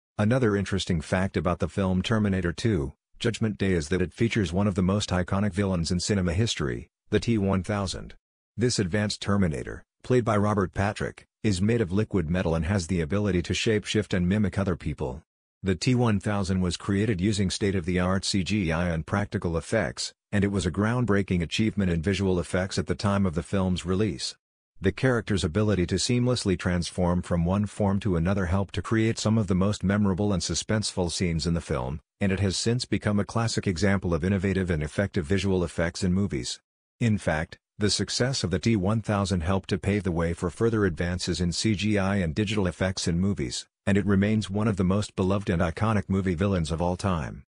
Terminator 2: Judgment Day's T-1000 cinema's most iconic villain
Another interesting fact about the film "Terminator 2: Judgment Day" is that it features one of the most iconic villains in cinema history: the T-1000. This advanced terminator, played by Robert Patrick, is made of liquid metal and has the ability to shape-shift and mimic other people. The T-1000 was created using state-of-the-art CGI and practical effects, and it was a groundbreaking achievement in visual effects at the time of the film's release. The character's ability to seamlessly transform from one form to another helped to create some of the most memorable and suspenseful scenes in the film, and it has since become a classic example of innovative and effective visual effects in movies. In fact, the success of the T-1000 helped to pave the way for further advances in CGI and digital effects in movies, and it remains one of the most beloved and iconic movie villains of all time.